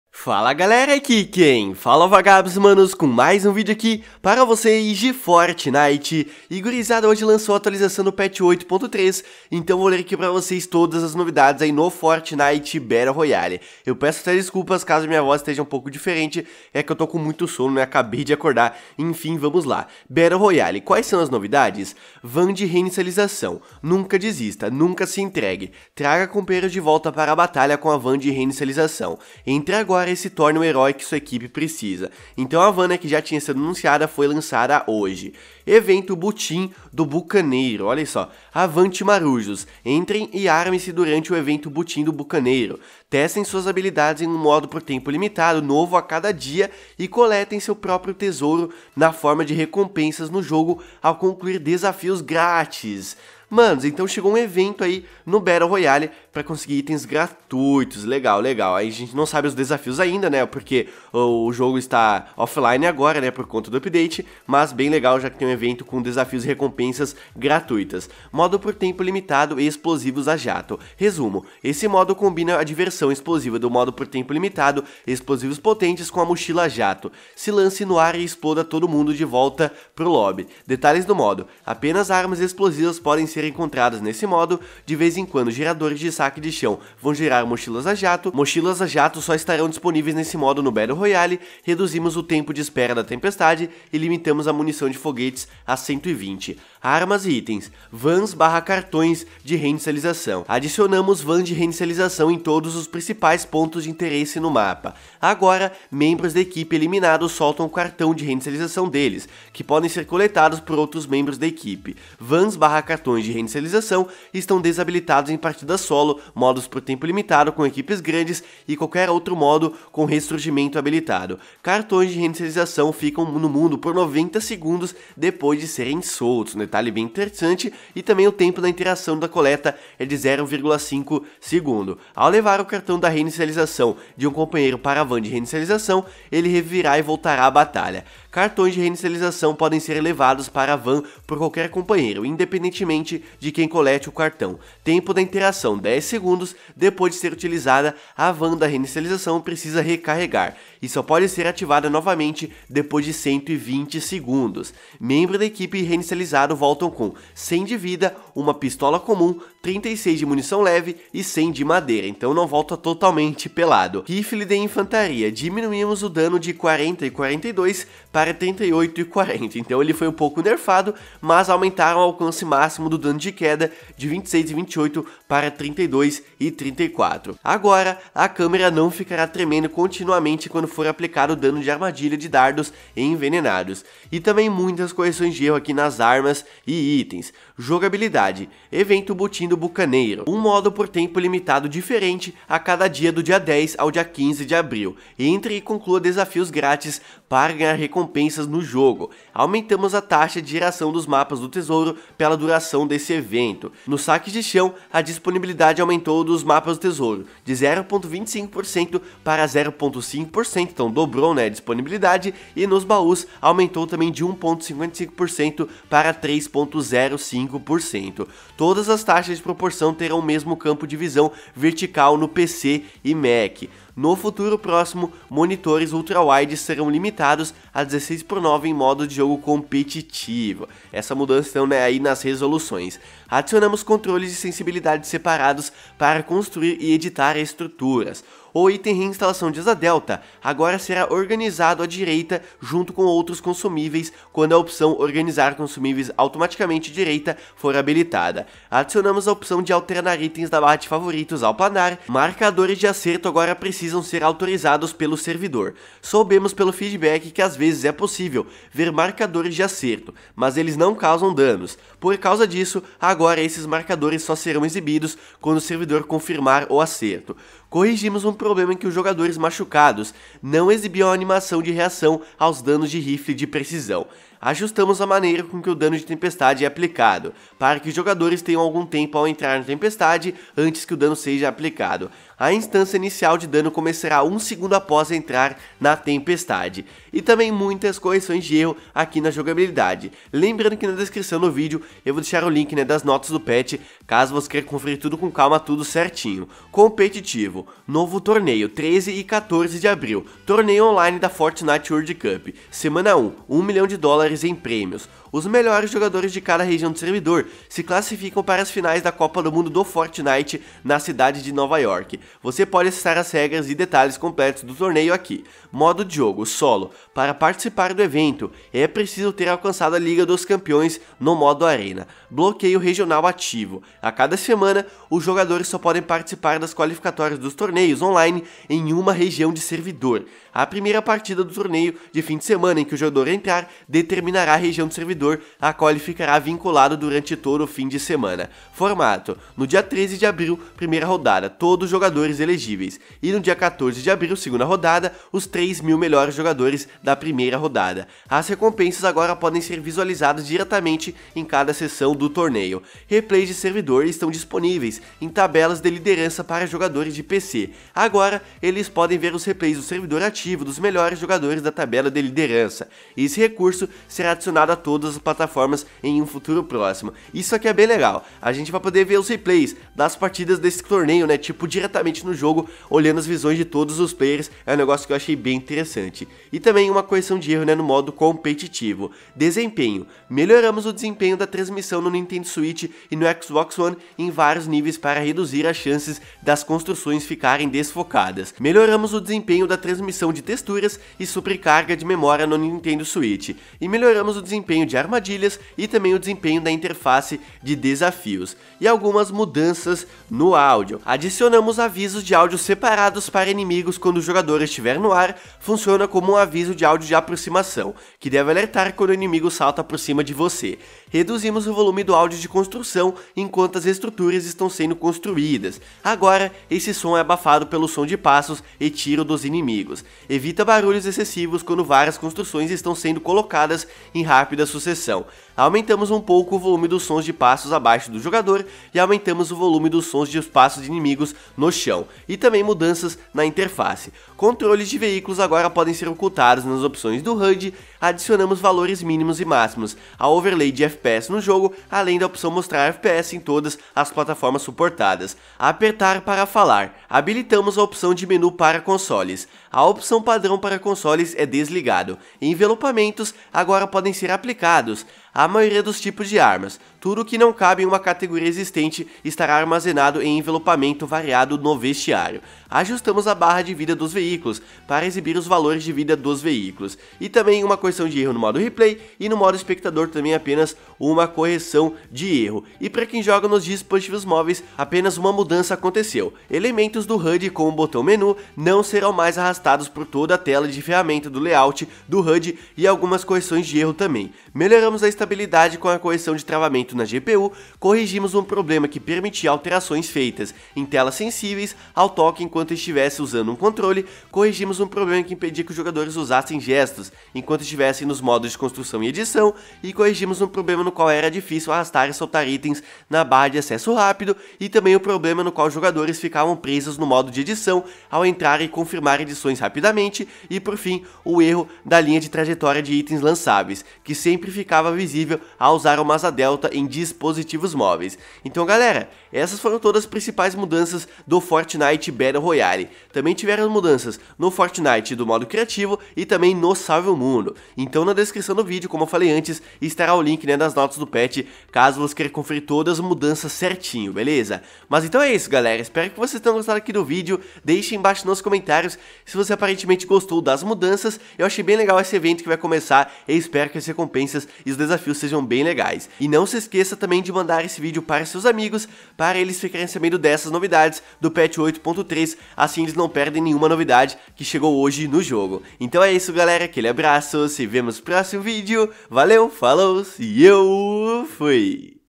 FORTNITE - NOVAS RECOMPENSAS, DESAFIOS e VAN DE REVIVER! - Patch 8.3
Fala gurizada, Vagabbss aqui com mais um vídeo, e dessa vez falando todas novidades do Patch 8.30 no Fortnite Battle Royale, como a Nova Van de Reinicialização, Novo Evento Butim do Bucaneiro com Desafios e Recompensas Grátis, e muito mais. Espero que gostem :)